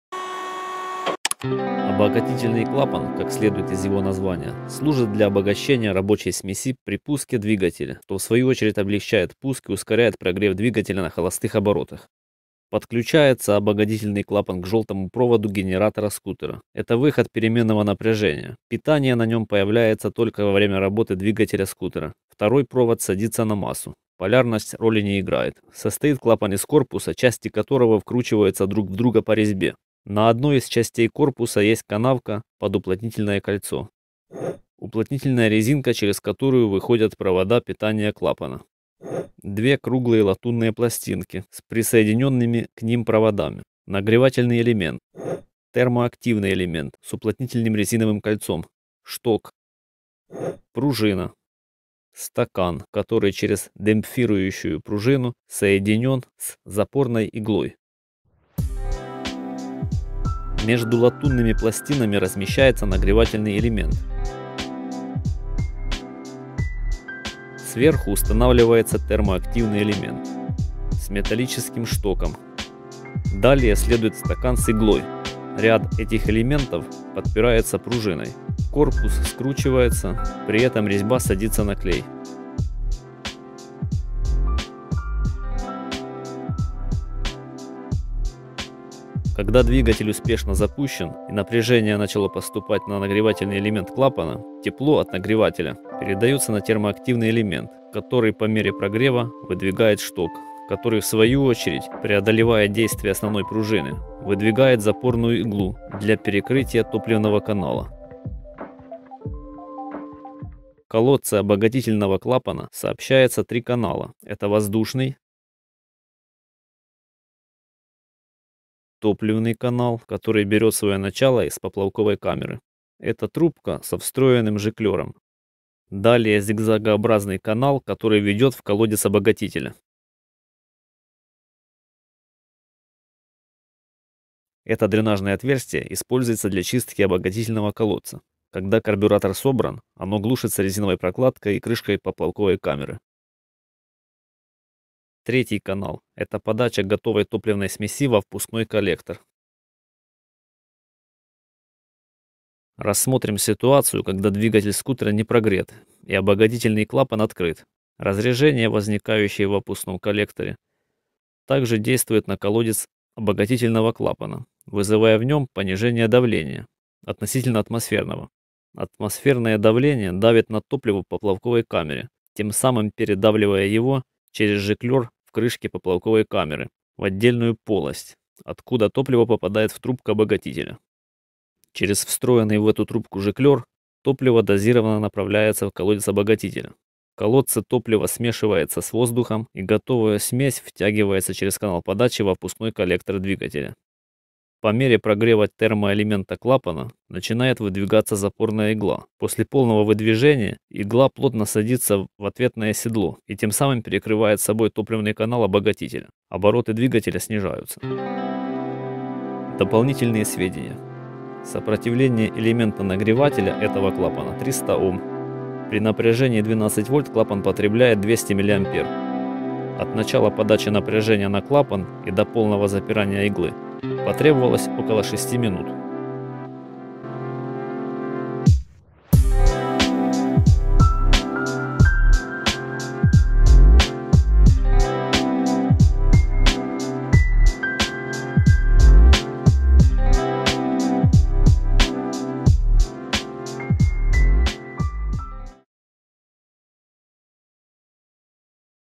Обогатительный клапан скутера | Как работает
Строение, принцип действия...
00:00 Вступление
00:25 Строение
01:28 Работа клапана
02:32 Работа карбюратора, во время прогрева
04:09 Характеристики данного клапана